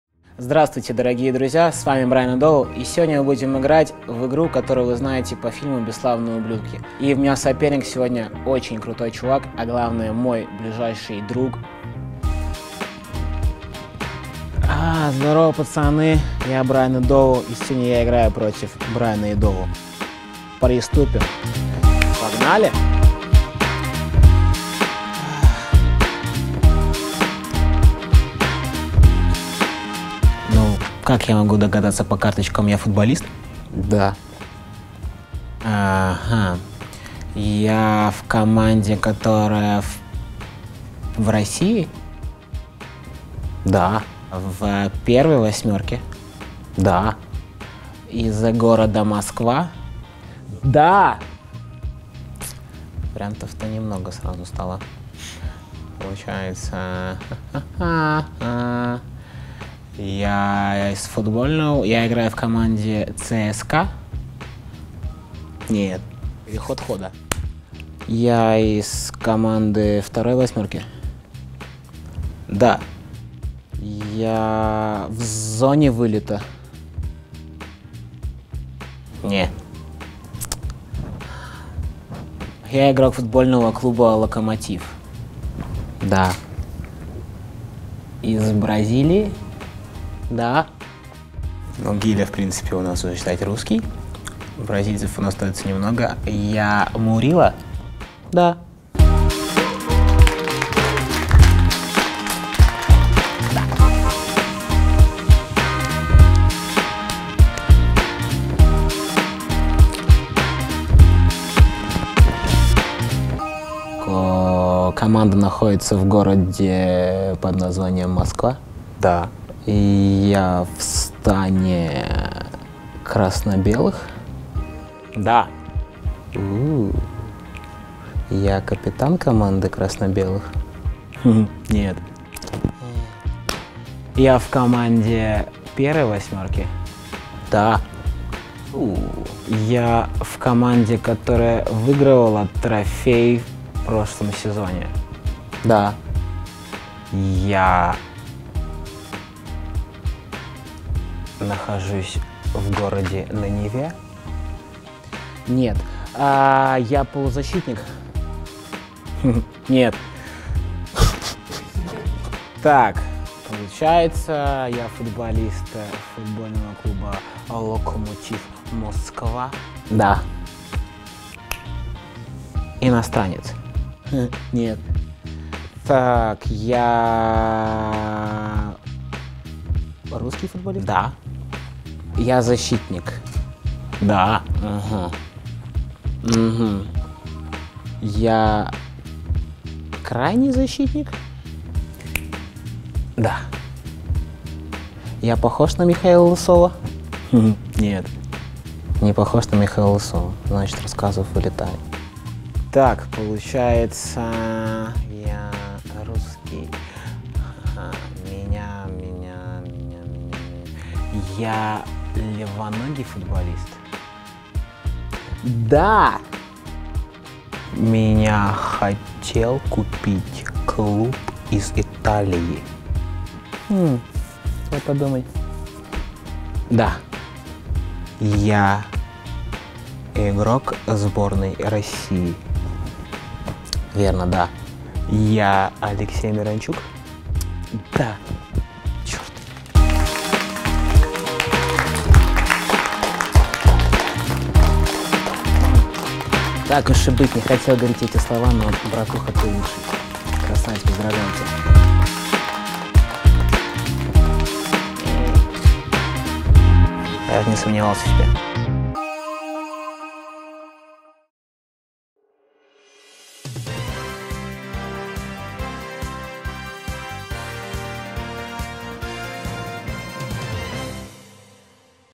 Баттл года // Идову VS Идову
Такого вы ещё не видели!

Баттл года: Брайан Идову vs Брайан Идову

Нет времени объяснять, просто посмотри новый ролик от LOKO ТВ

Спойлер: победил Брайан Идову!